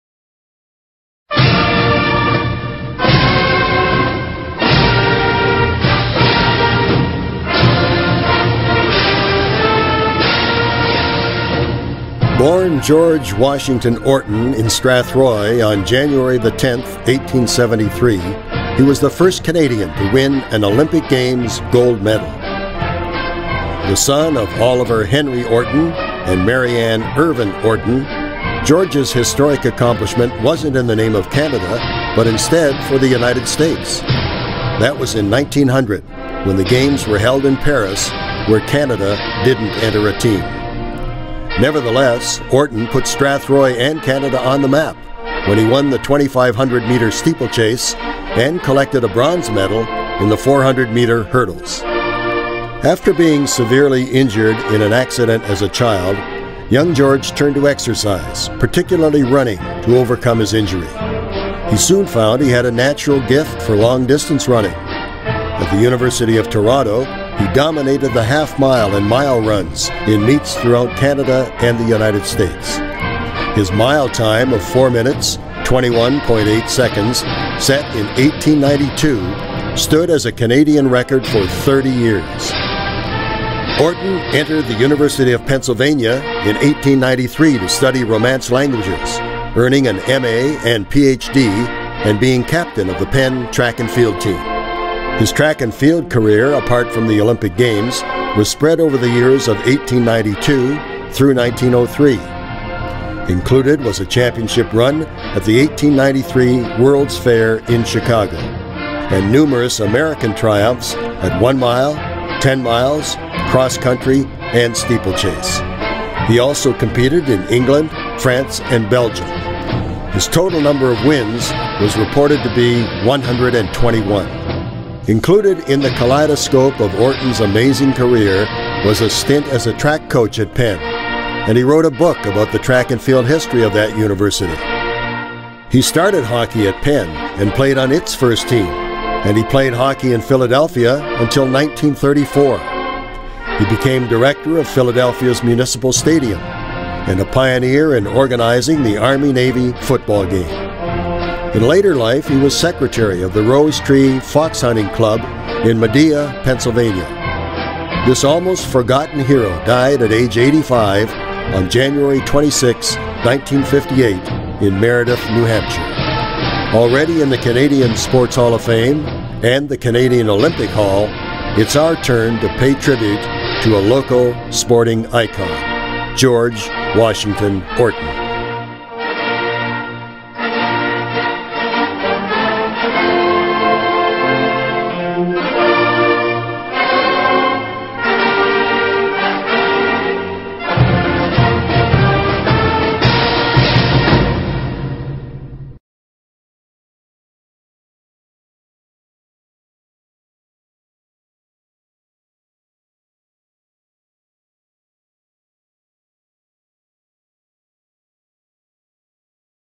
GEORGE ORTON
Born George Washington Orton in Strathroy on January 10, 1873, he was the first Canadian to win an Olympic Games gold medal.
The son of Oliver Henry Orton and Mary Ann Irvine Orton, George's historic accomplishment wasn't in the name of Canada but instead for the United States. That was in 1900 when the Games were held in Paris where Canada didn't enter a team.
Nevertheless, Orton put Strathroy and Canada on the map when he won the 2,500-metre steeplechase and collected a bronze medal in the 400-metre hurdles.
After being crippled in an accident as a child young George turned to exercise, particularly running, to overcome his injury. He soon found he had a natural gift for long distance running. At the University of Toronto he dominated the half-mile and mile runs in meets throughout Canada and the United States. His mile time of 4:21.8 set in 1892 stood as a Canadian record for 30 years.
Orton entered the University of Pennsylvania in 1893 to study romance languages earning an M.A. and PhD and being captain of the Penn track and field team. His track and field career apart from the Olympic Games was spread over the years of 1892 through 1903. Included was a championship run at the 1893 World Fair in Chicago and numerous American triumphs at one-mile, 10 miles, cross-country, and steeplechase. He also competed in England, France and Belgium. His total number of wins was reported to be 121.
Included in the kalidescope of Orton's amazing career was a stint as a track coach at Penn and he wrote a book about the track and field history of that university. He was also an outstanding athlete in other sports. He had played on the U of T soccer team and was still playing soccer in Philadelphia for the Merchantville team in 1923.
He started hockey at Penn and played on its first team. And he played hockey in Philadelphia until 1934. He became director of Philadelphia's Municipal Stadium and a pioneer in organizing the Army-Navy football game. He founded a youth camp and playground association and authored books for boys. In later life he was secretary of the Rose Tree Fox Hunting Club in Media, Pennsylvania. And he dabbled in poetry.
This almost forgotten hero died at age 85 on January 26, 1958 in Meredith, New Hampshire. Already in the Canadian Sports Hall of Fame and the Canadian Olympic Hall, it's our turn to pay tribute to a local sporting icon.